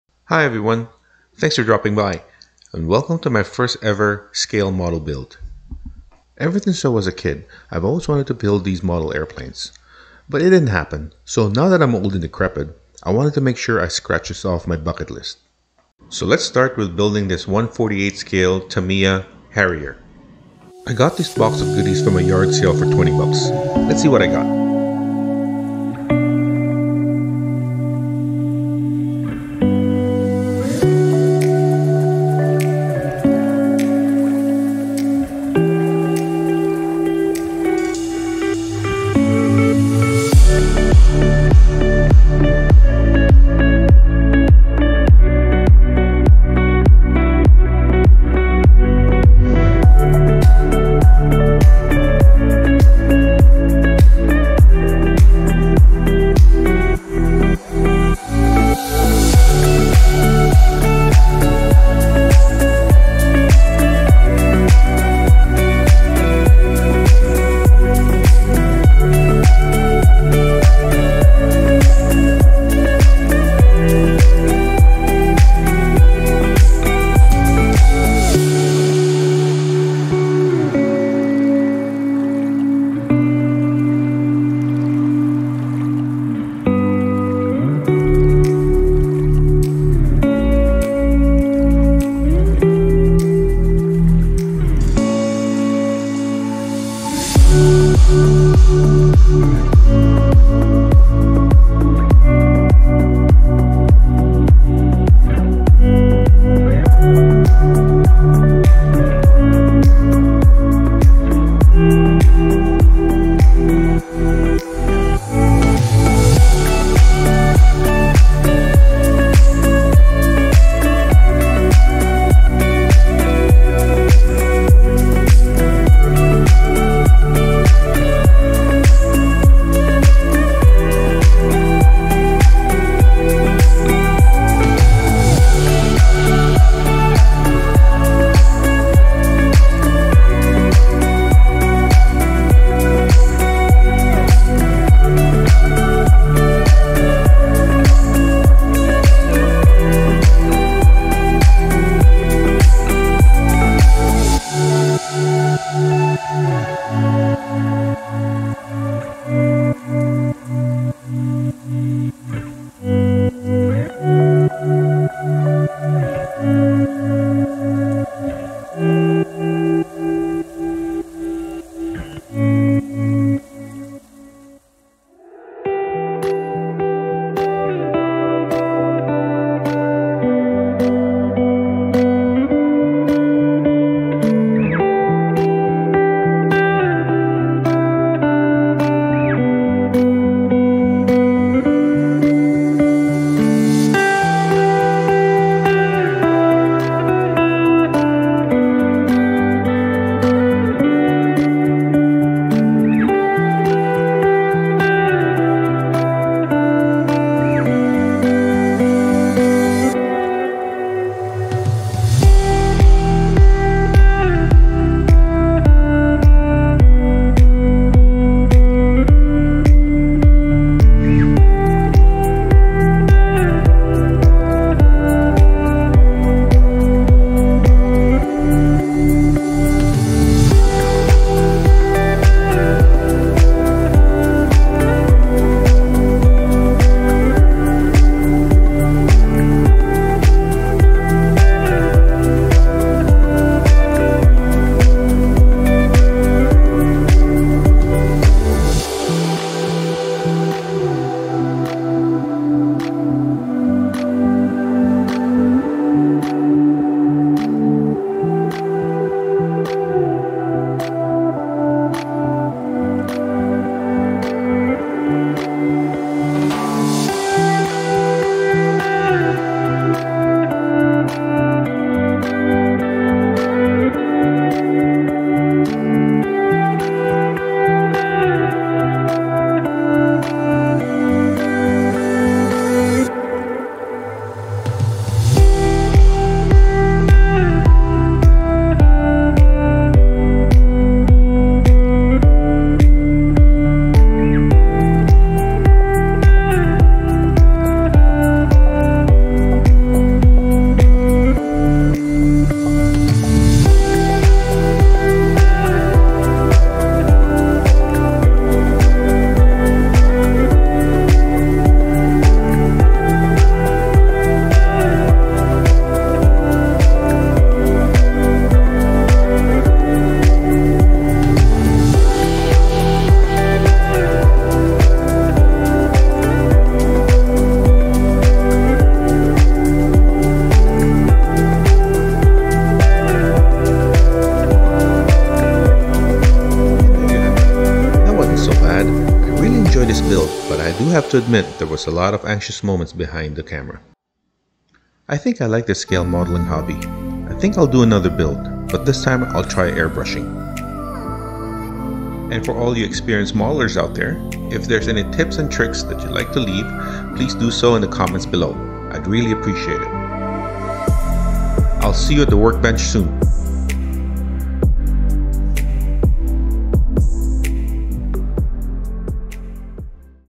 My First Build | Tamiya 1/48 Royal Navy Sea Harrier FRS.1 | Hand Brush
I finally did it.  Built a model kit from scratch!  something I've always wanted to do as a child.